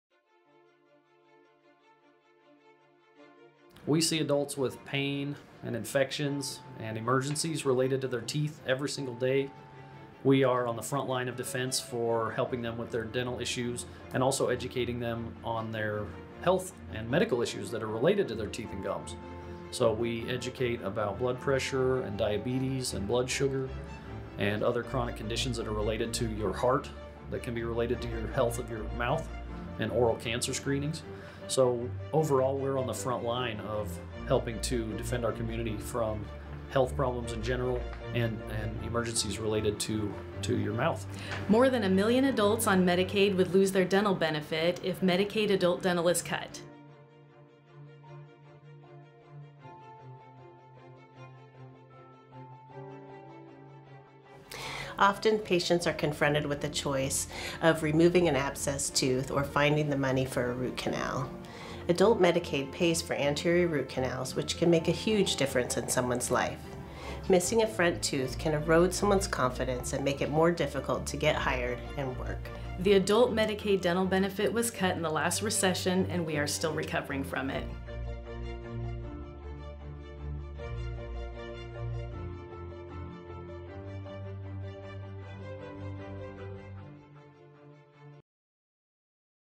Protect the Adult Dental Medicaid Benefit!
Unity Care NW dental leadership asks WA state legislators to continue the adult dental Medicaid benefit. Cutting the programs will only lead to much larger, and more expensive problems for people who are already struggling. Oral health issues, if left untreated, can have serious consequences on chronic conditions such as diabetes and is linked to heart disease, stroke, pneumonia, and pregnancy complications.